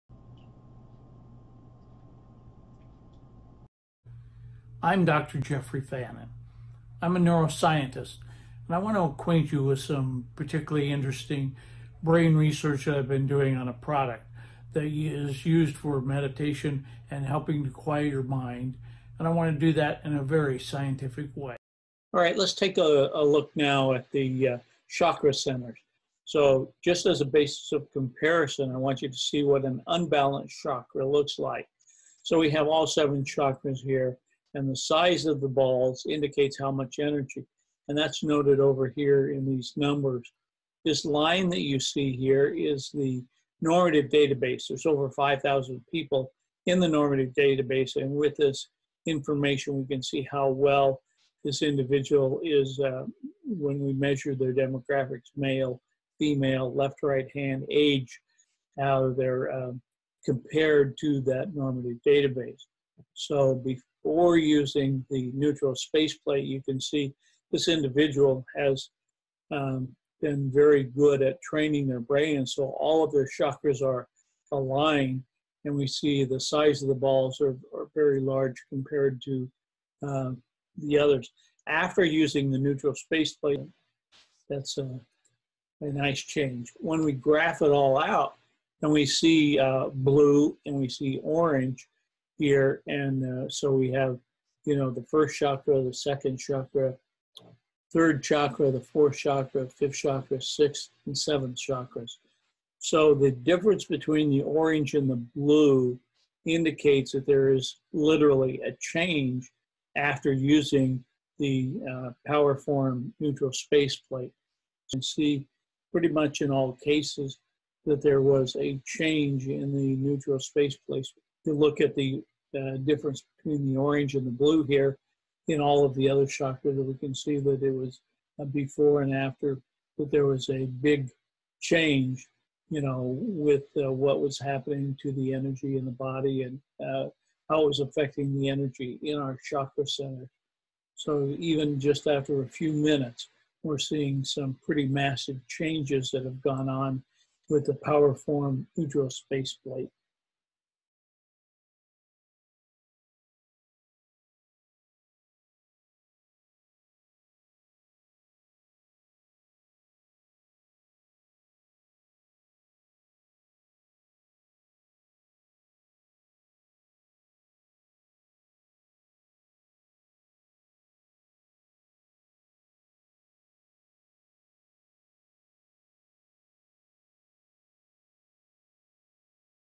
Powerforms & Chakras
Powerforms & Chakra research.  Dr. Jeffrey Fannin Ph.D. neuroscientist explores the brain before and after using Powerforms